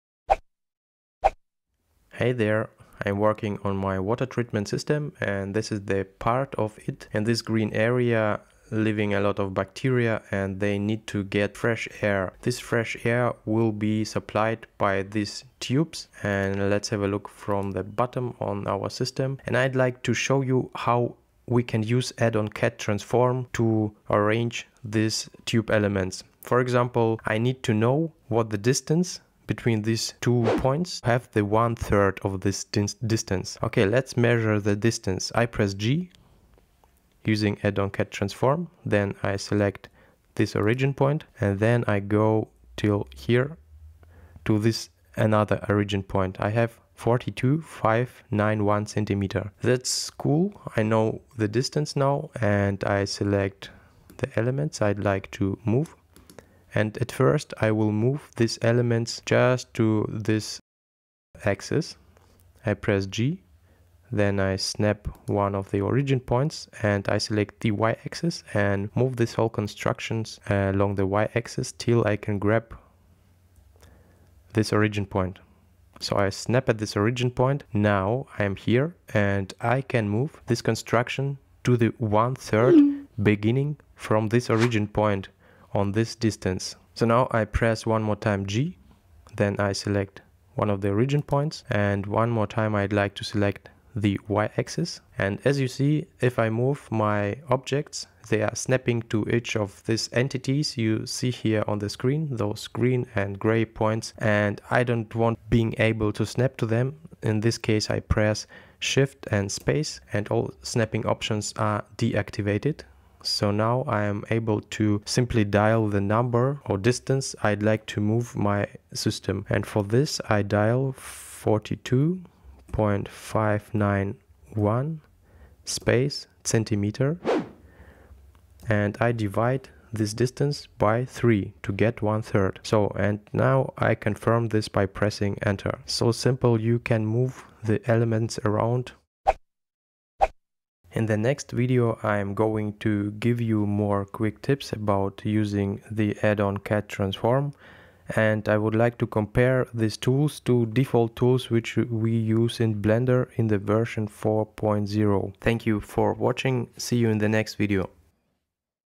UPDATED CAD Transform 2.0 for Blender 4.+
I am currently using the latest version of the CAD Transform Add-on, created by the maker of the impressive ArchiPack add In this and future short videos, I aim to kick off a quick tips guide on various techniques and tricks for applying CAD Transform. The series will demonstrate, how to utilize a custom coordinate system and toggle the snap system on and off and much more in further videos. This video will show you a really simple method for precise movement.

In this video, I am using a water treatment system construction for illustration purposes. Once I complete this project, you'll be able to download the Blender file. I'm making an effort to keep the construction as light weight as possible in terms of amount of vertices.